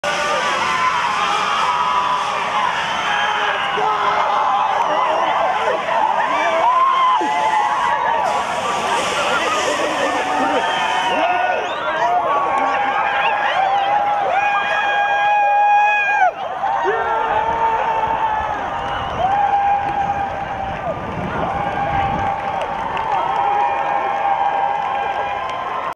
Freshmen Run Out! Samford Family Weekend!
The Class of 2016 got things started at Samford's football game Saturday!  A day that saw the Bulldogs beat the 6th ranked Wofford Terriers.